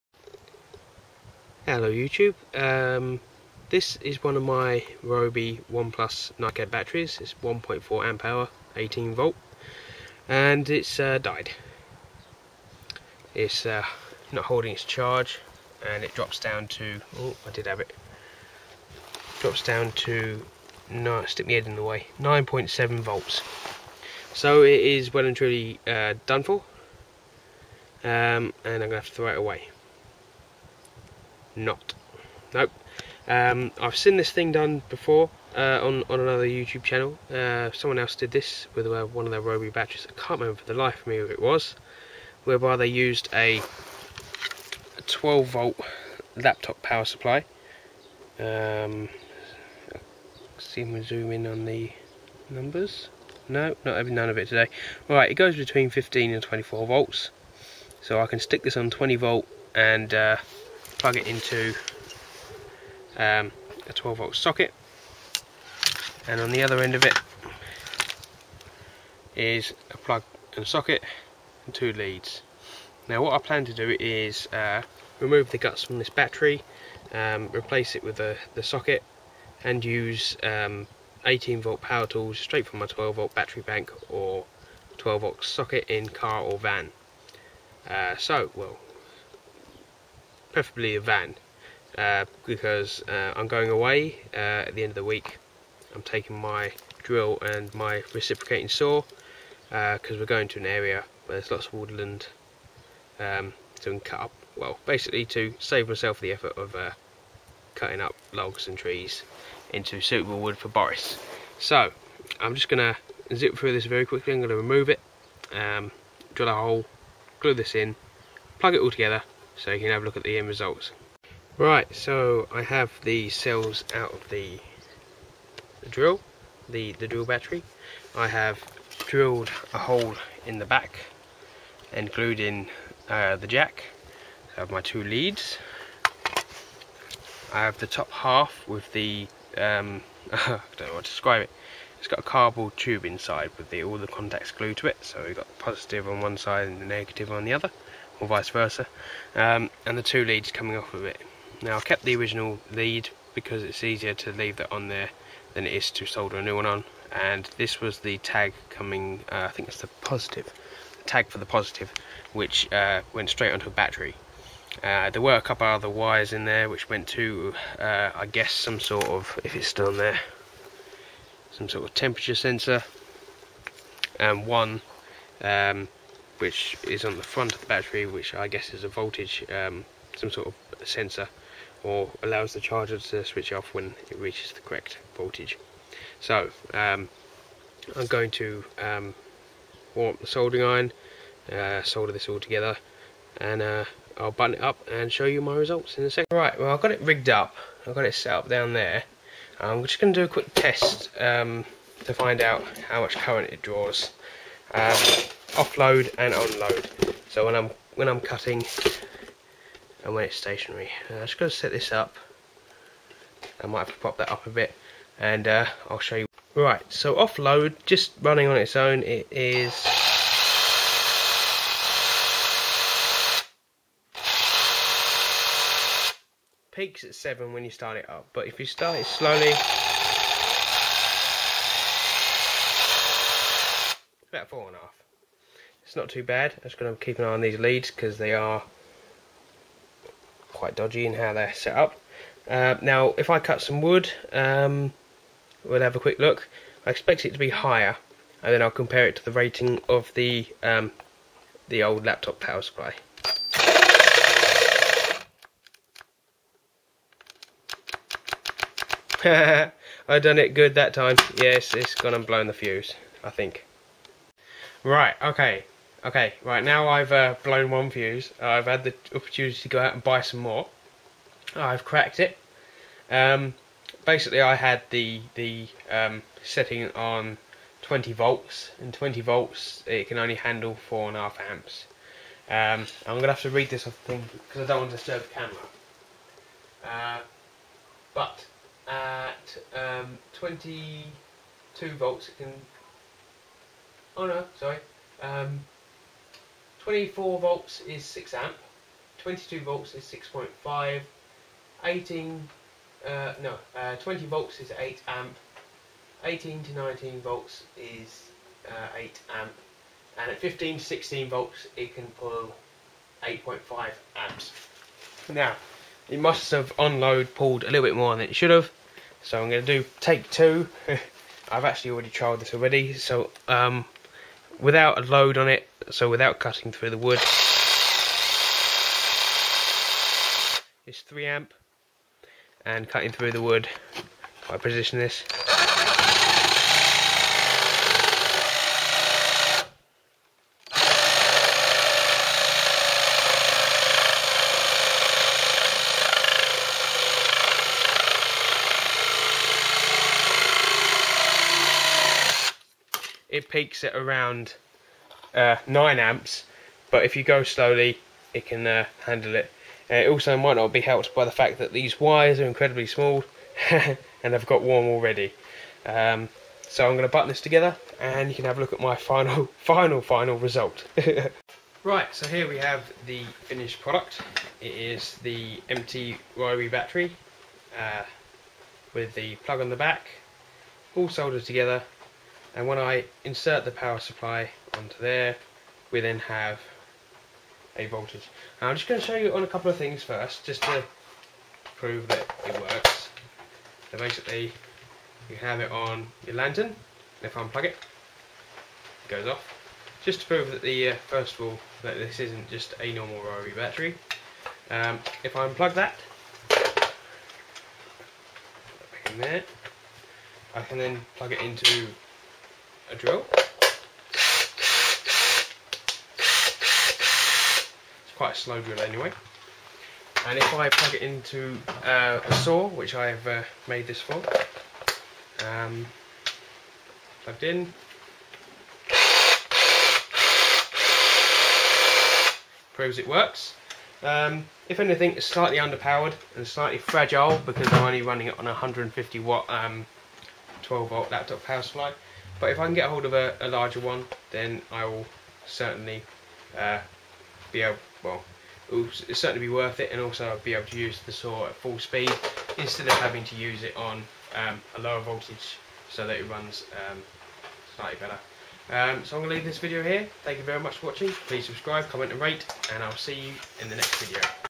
Ryobi battery conversion
Had a Ryobi 18V one plus battery fail. So i ripped it apart and used a 12V laptop power supply to run the dummy battery case!